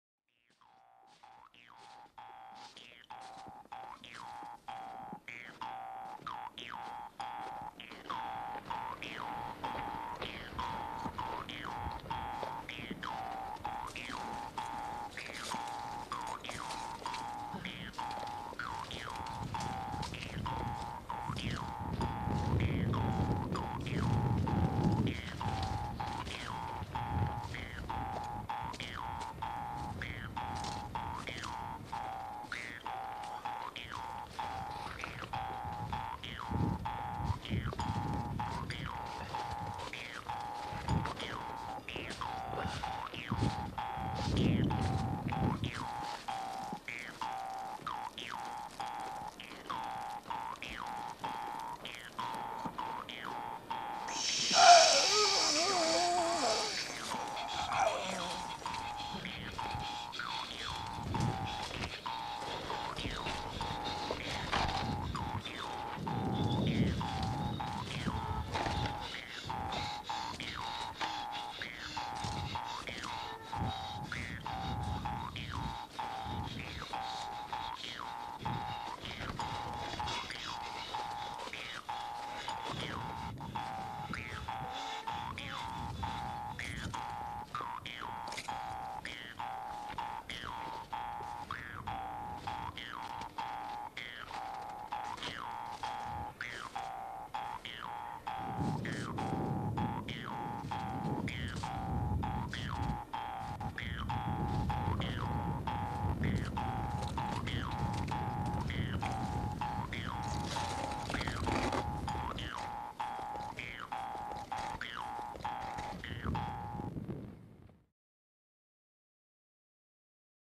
Mooncatcher
A artvideo about a man who try to catch the moon